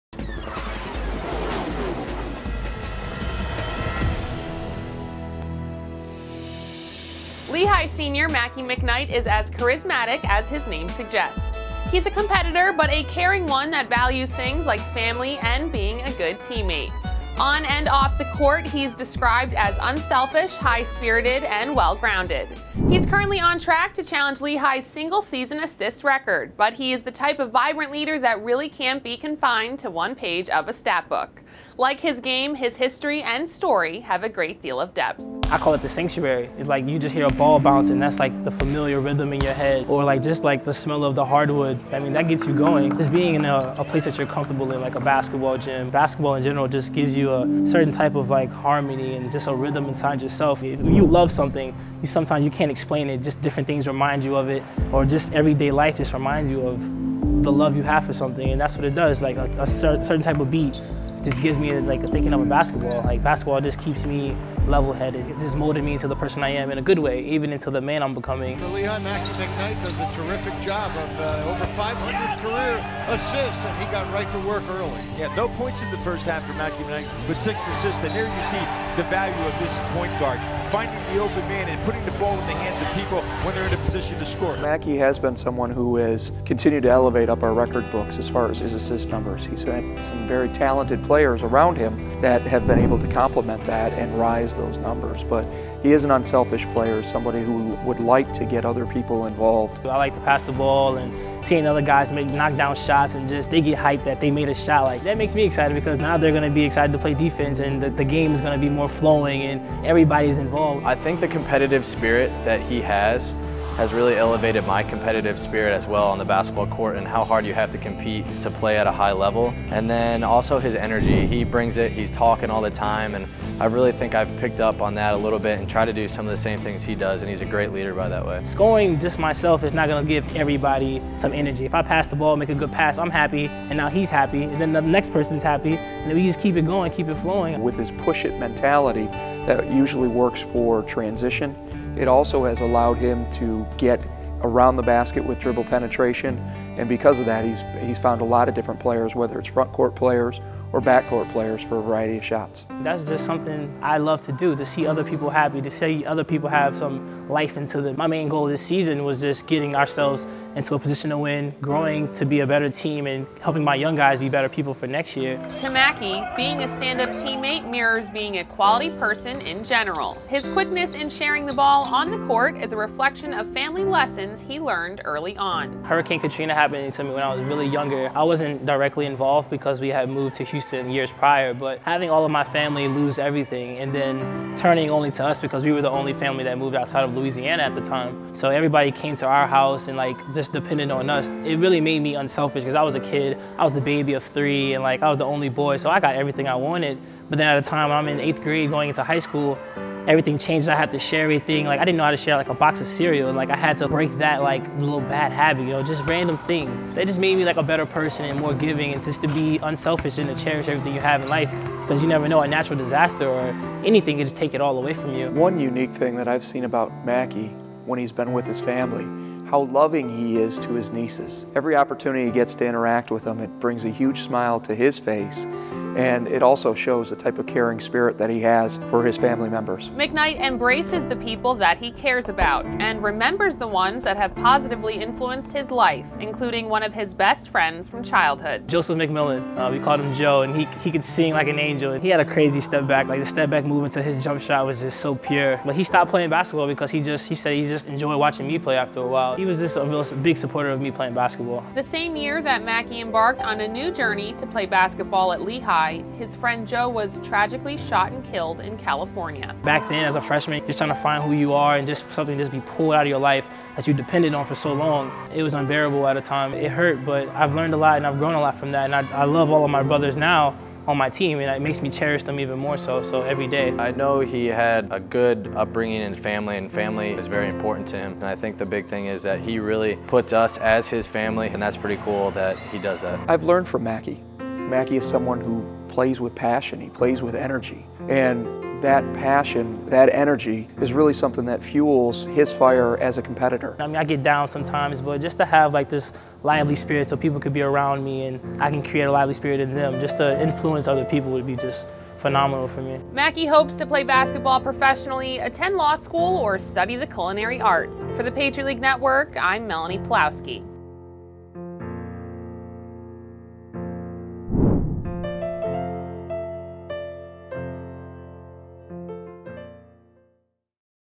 Mountain Hawks Have Spirited Leader in McKnight
Lehigh senior guard Mackey McKnight plays with a certain passion and energy on the court that mirrors his positive outlook on life.